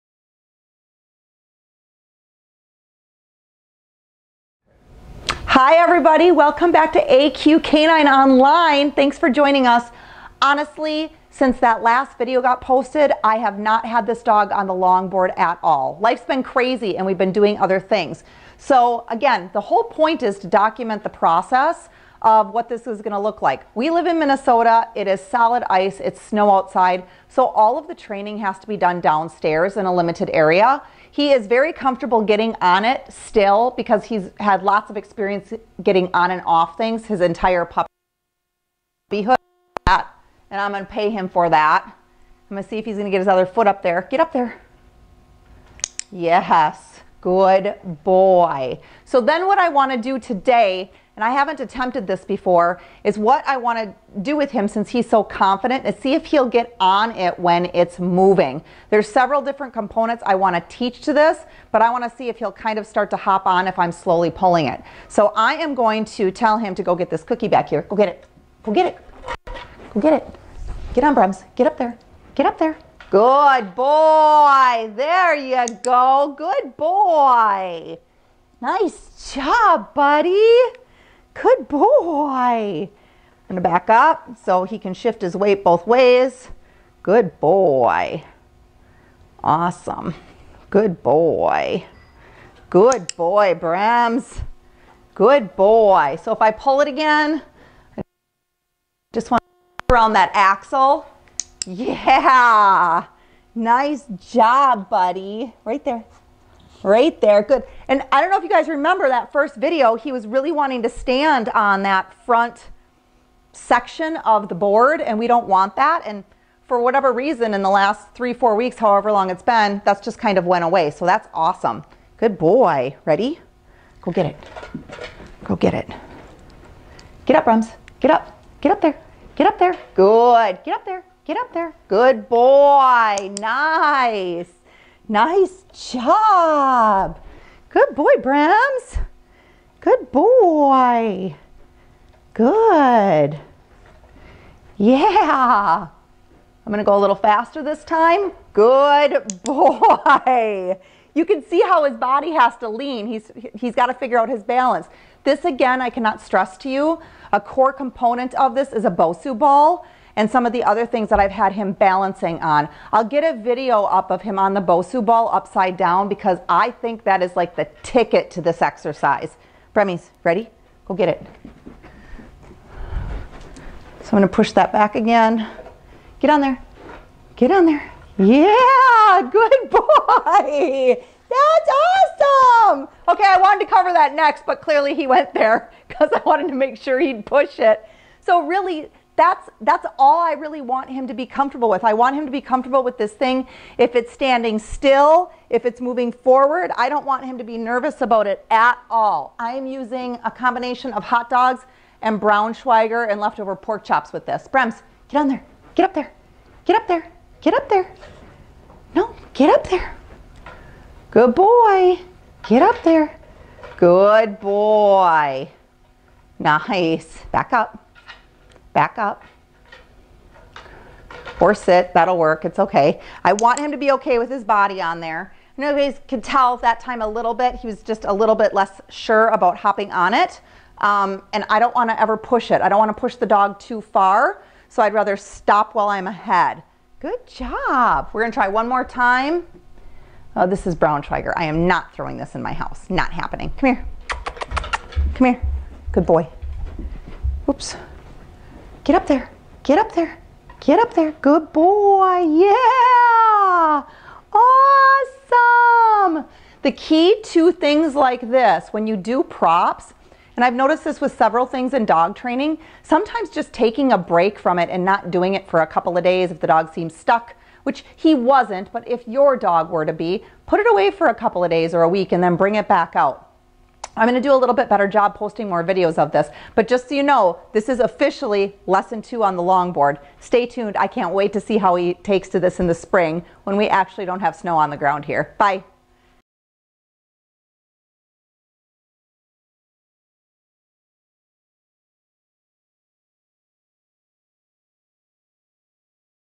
Amstaff - Longboarding 2
Brembo is an eight month old American Staffordshire Terrier.  We are introducing him to longboarding.  We will cover his journey from learning to stand properly, balance while moving, getting the board moving, turning and getting off the board.

Brembo is already well trained and has been introduced to a lot of different environments.

Follow his journey of learning.  He should be ready to hit the road when the snow melts.